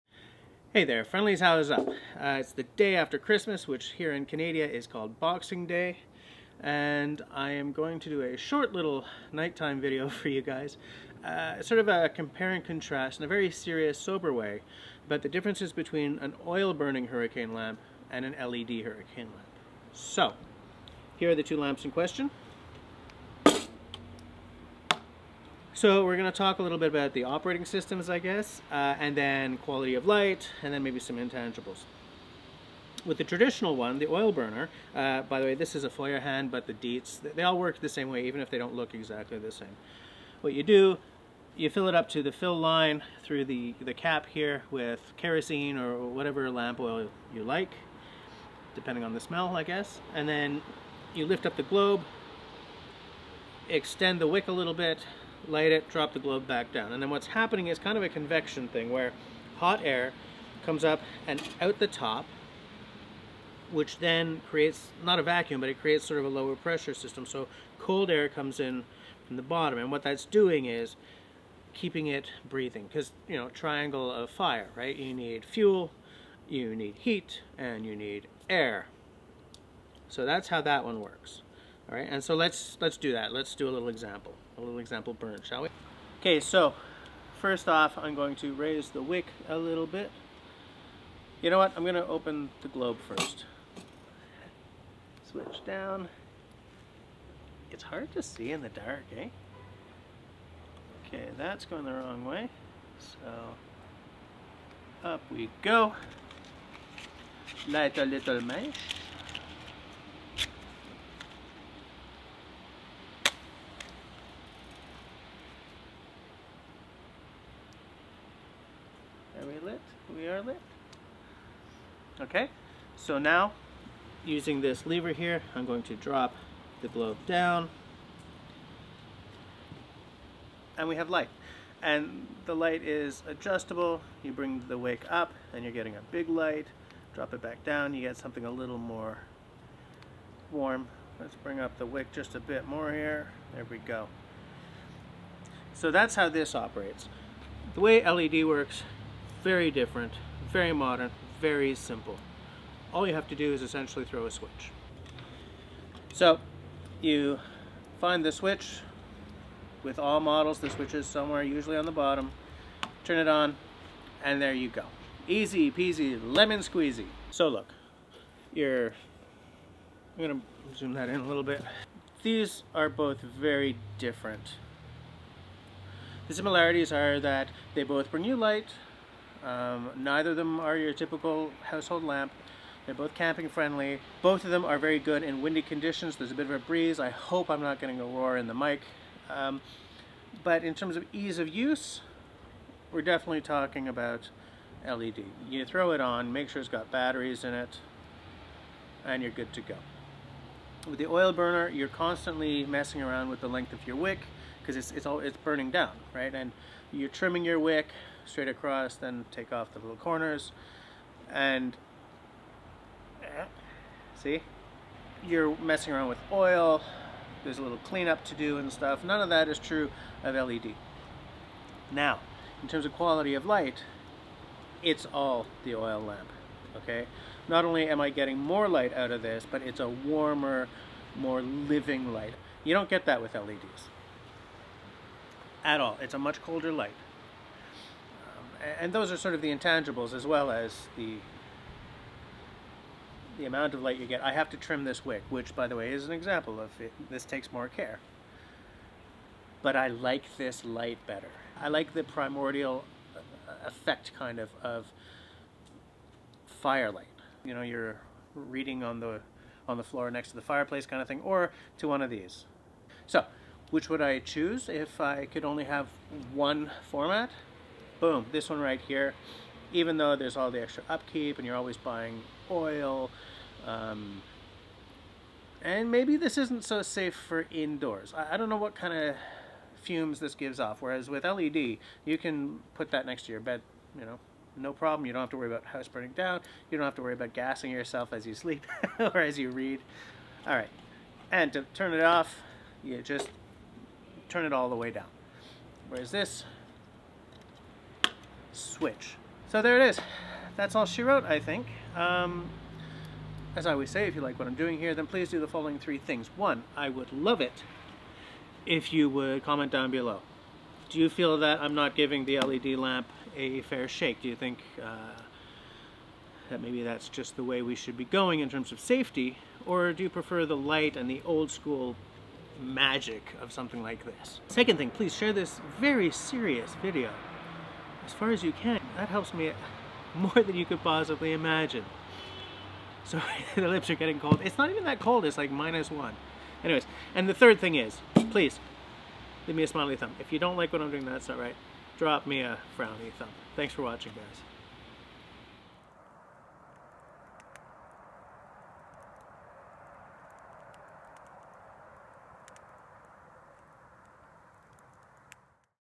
Hurricane lanterns: oil or LED? a totally, completely, way serious comparison
Hurricane lanterns, oil or LED? Here's a comparison between the two, going over operating systems, light quality and the intangibles.

Best viewed in 1080p | This video does not contain sponsored content.

My Facebookz!

My Instagramz!